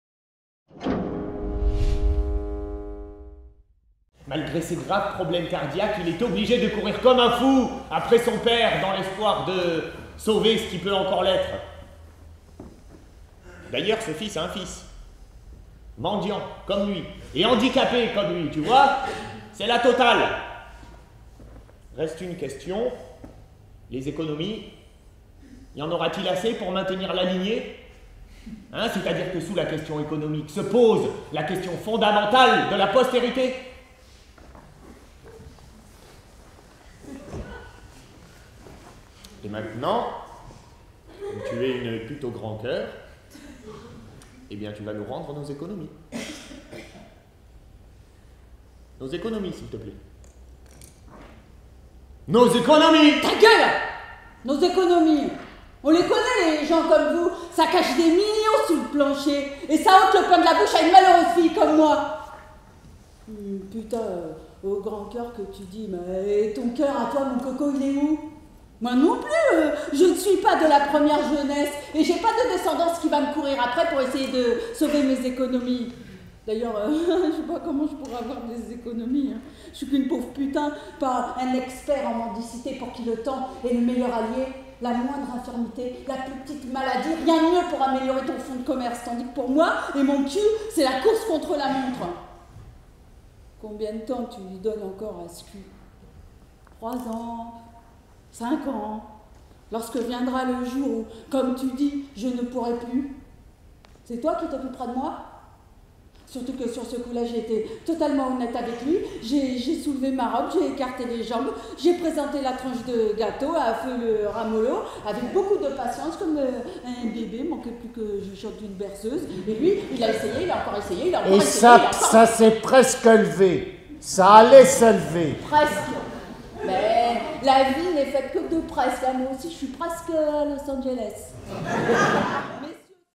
Pleins Feux - "La putain de l'Ohio"
- Pleins feux - Un court extrait sans montage pour vous faire découvrir cette pièce sans trop en dévoiler !